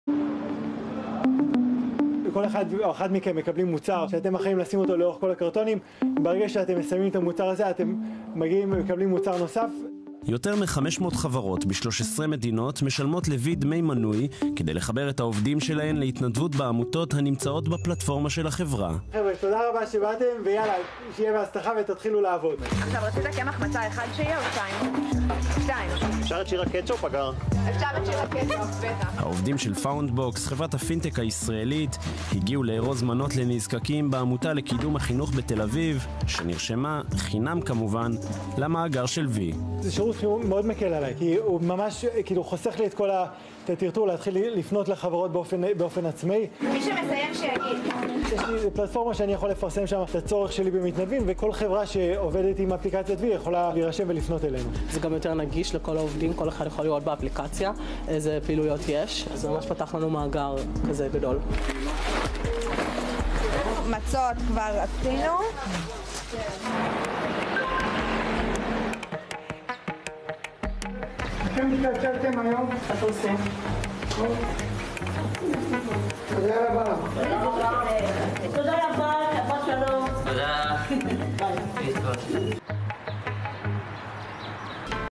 מרכז לחלוקת מזון והתנדבות - העמותה לקידום החינוך בת"א-יפו The Food Distribution Center & volunteers
תכונה בעברית על מתנדבים ומרכז לחלוקת מזון על קאן ניוז